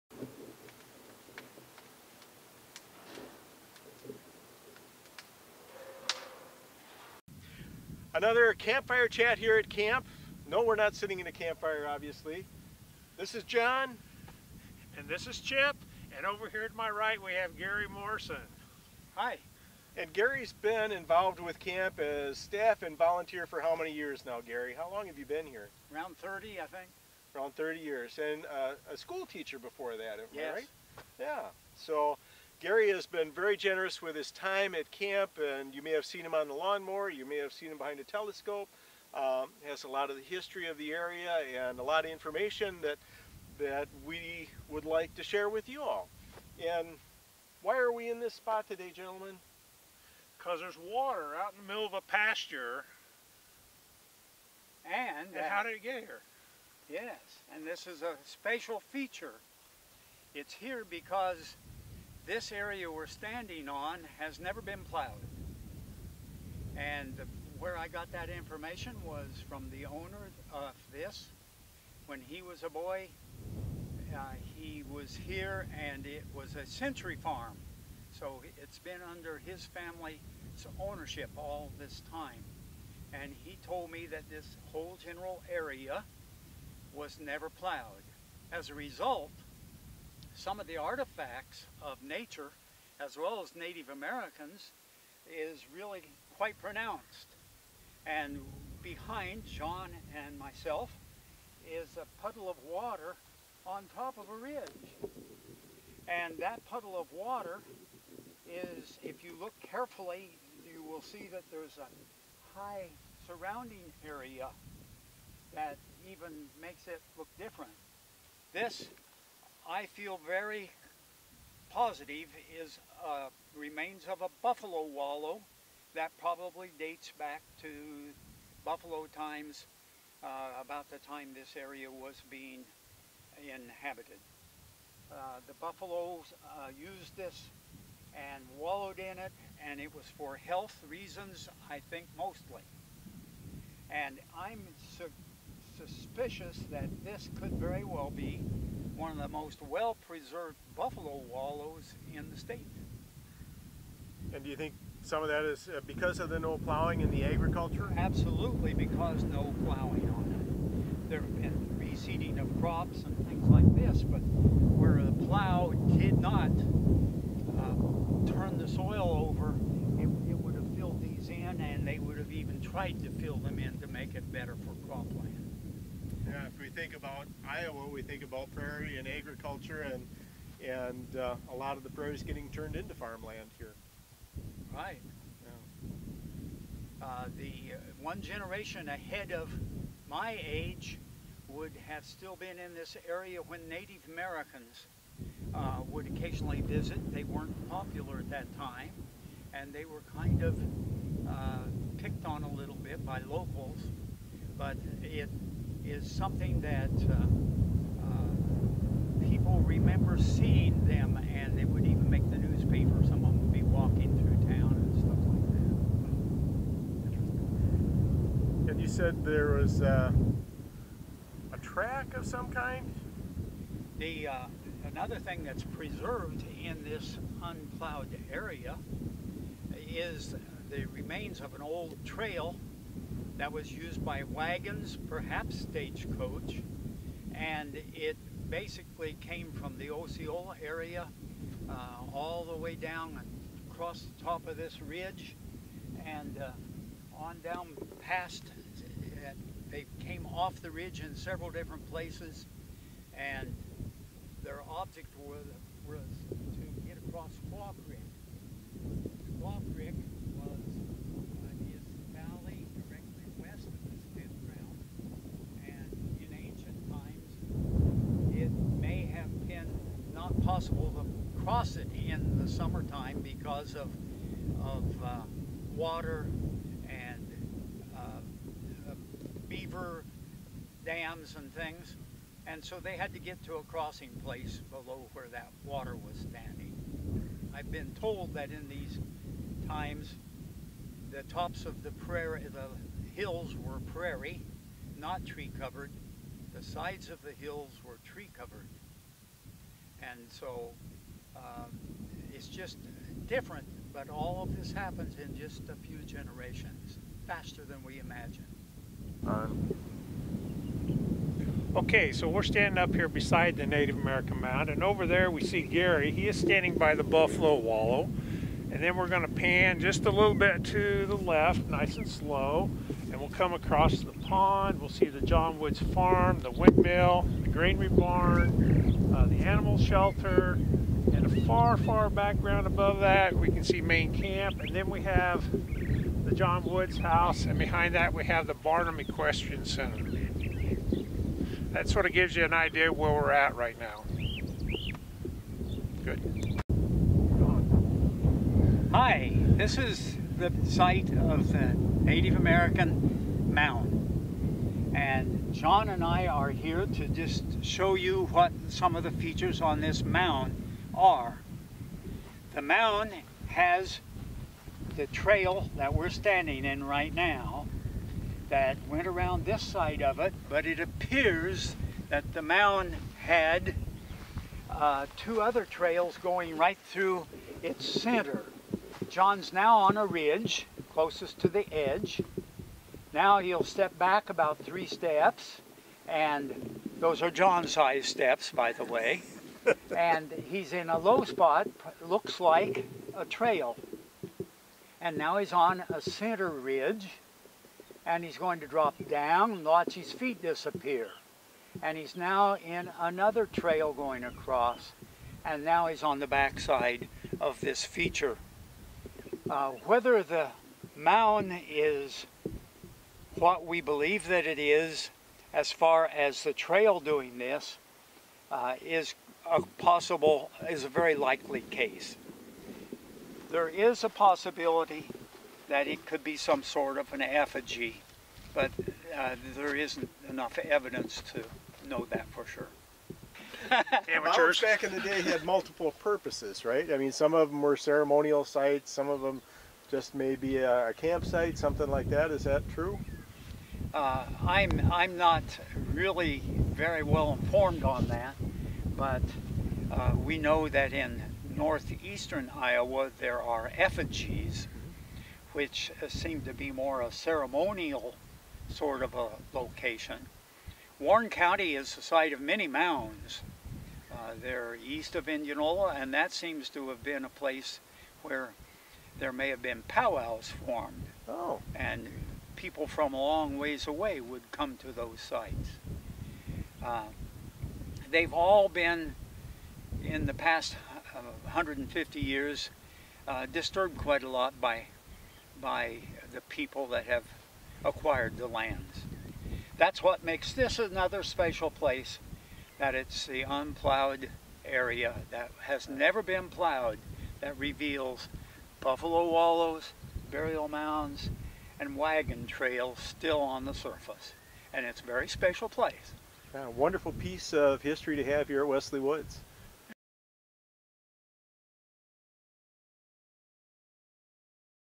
CC Buffalo Wallow
Join us in the South pasture as we talk about some of the history of the grounds prior to Wesley Woods.